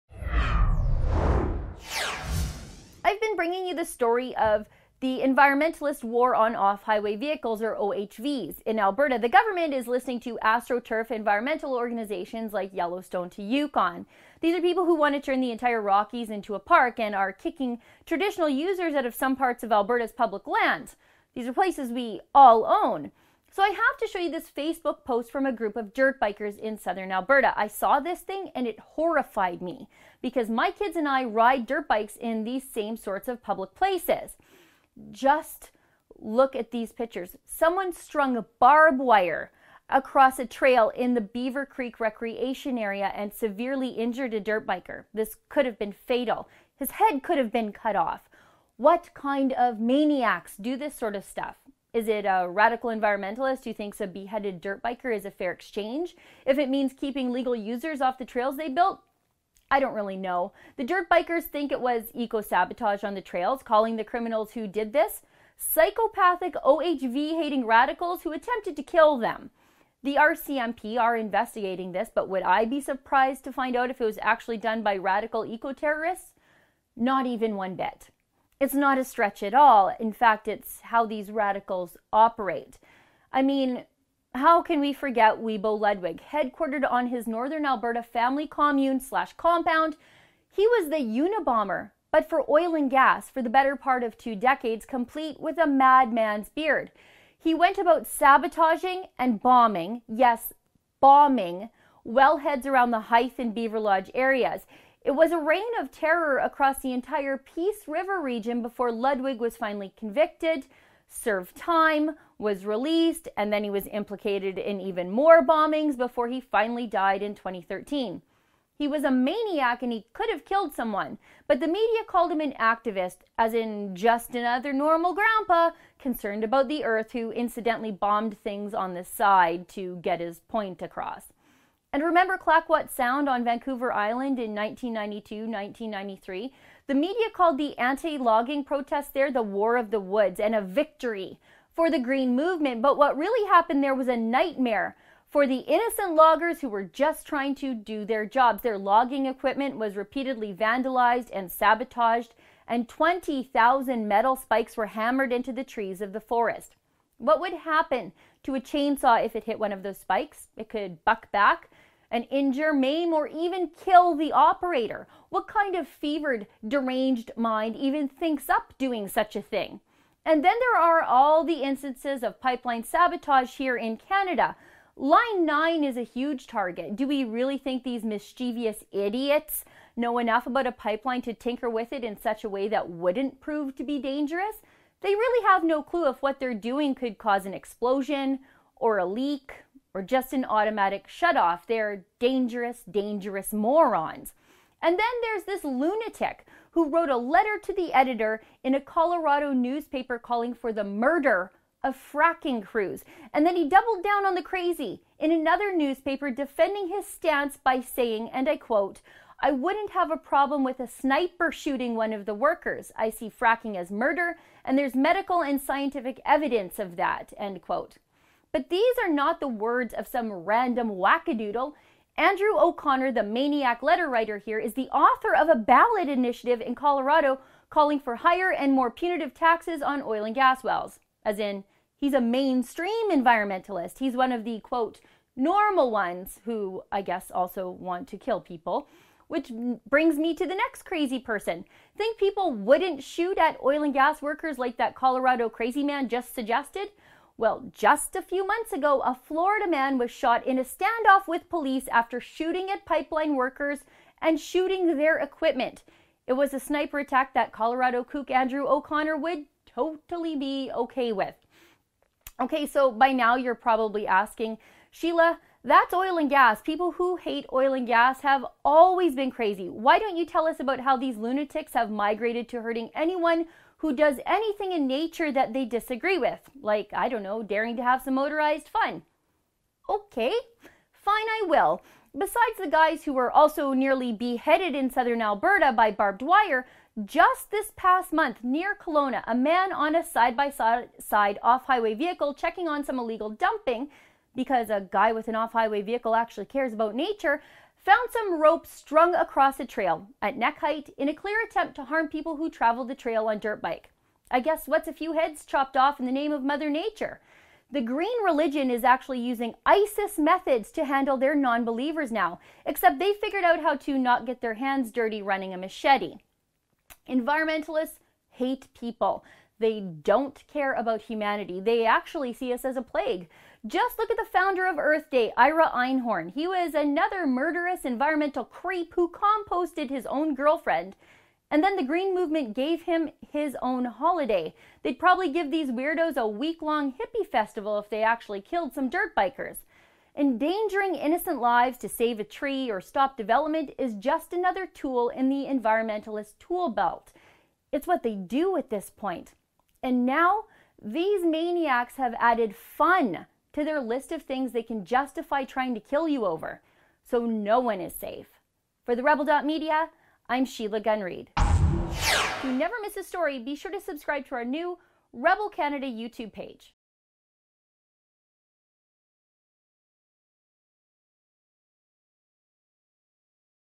Sabotage? Eco-radical war on OHVs takes deadly turn
Sheila Gunn Reid of TheRebel.media reports a potentially deadly incident in the war on off-highway vehicles that some suspect may be a case of trail sabotage. MORE

The Petition
Rachel Notley and the Alberta NDP government must keep their promise and drop their ban on off-highway vehicles in Castle Parks.